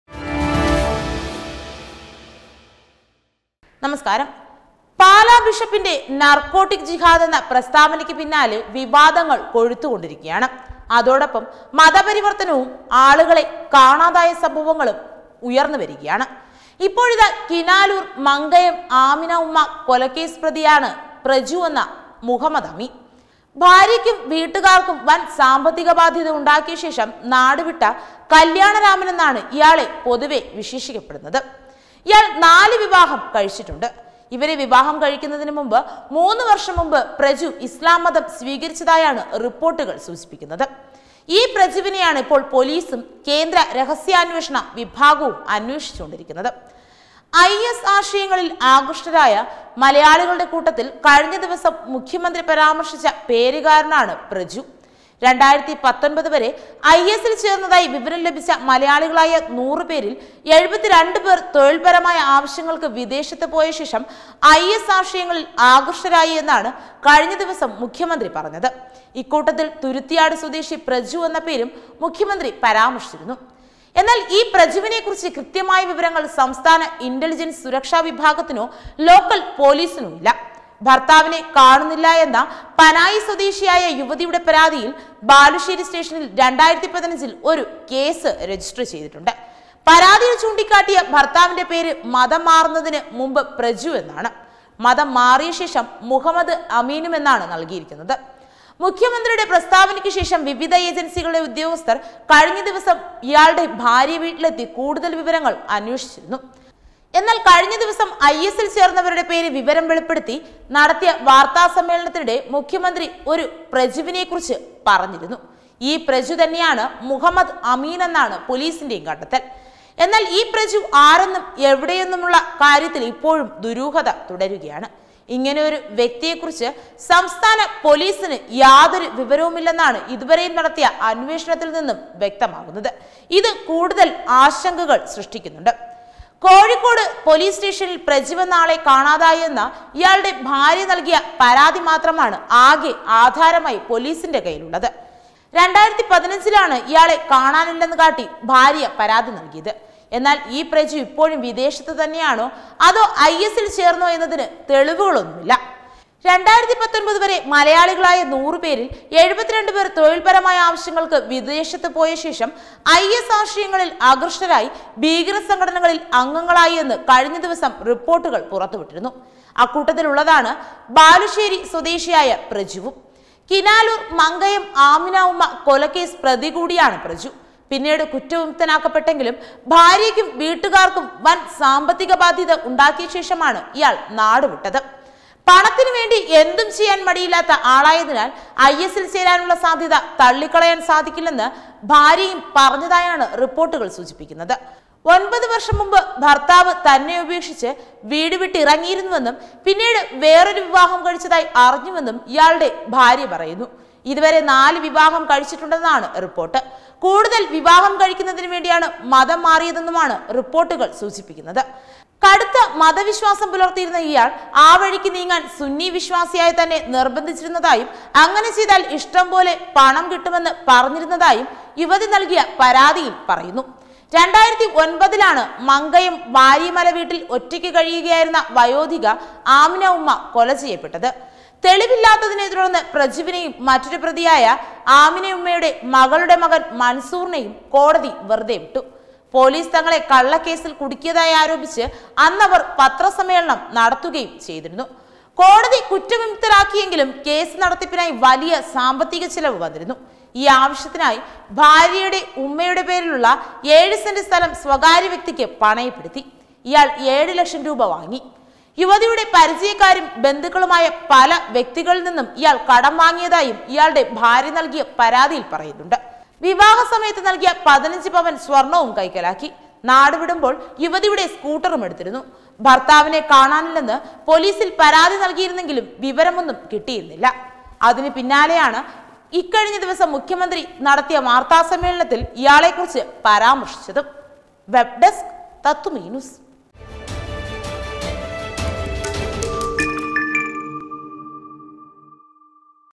പ്രജു എന്ന കള്ളപ്പേരിൽ നടന്ന ലൗ ജിഹാദിലെ മുഖ്യകണ്ണി "മുഹമ്മദ് അമീൻ'' എവിടെ? | PRAJU
പ്രജു എന്ന "മുഹമ്മദ് അമീൻ' ലൗ ജിഹാദിലെ മറ്റൊരു കണ്ണിയോ ? | PRAJU